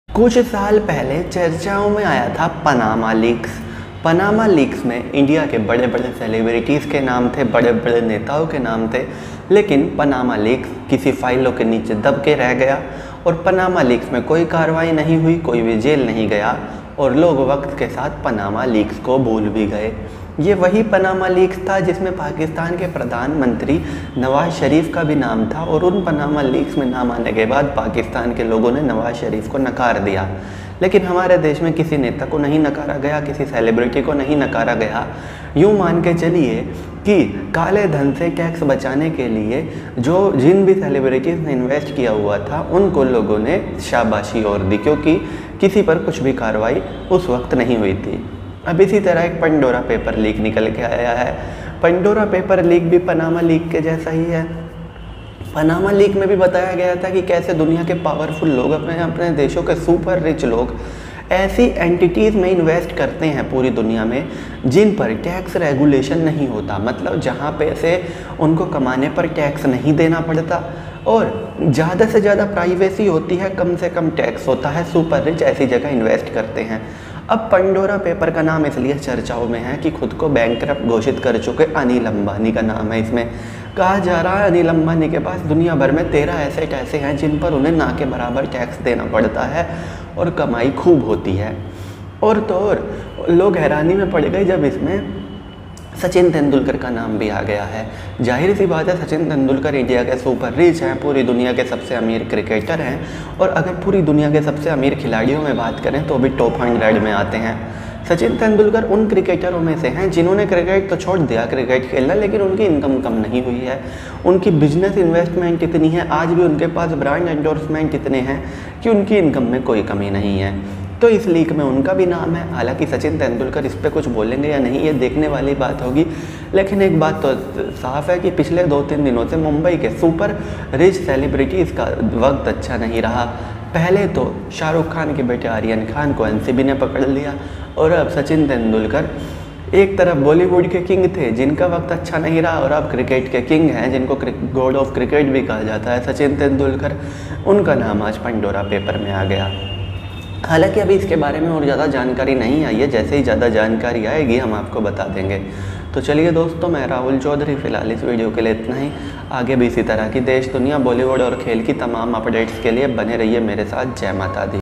क्या है Pandora Papers जिनमे Sachin Tendulkar, Anil Ambani का नाम आया है
Sachin tendulkar name emerged in pandora papers.